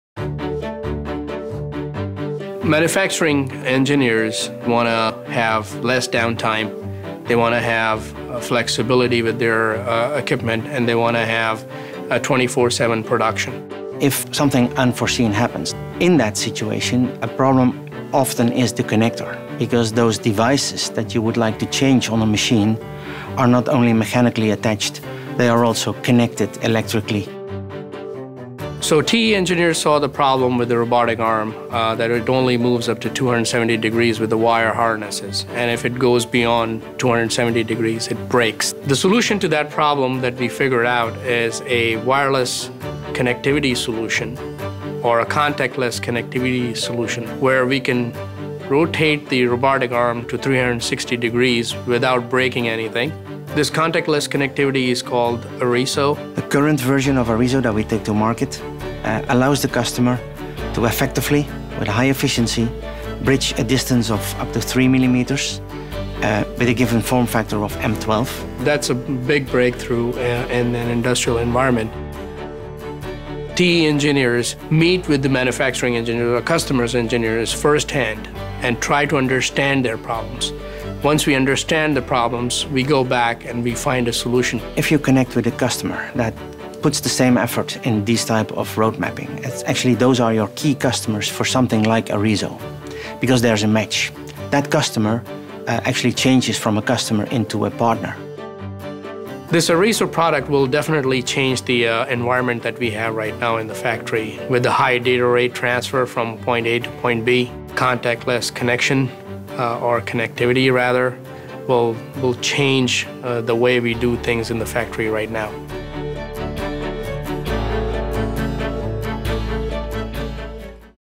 TE Connectivity: Automation & Control Technologies
Traditional designs limit robotic arm's motion to 270 degrees, due to wiring harnesses. TE Connectivity developed ARISO contactless connectors, enabling full 360-degree motion and require no wired harness. New arm rotations are now possible, opening up new manufacturing possibilities.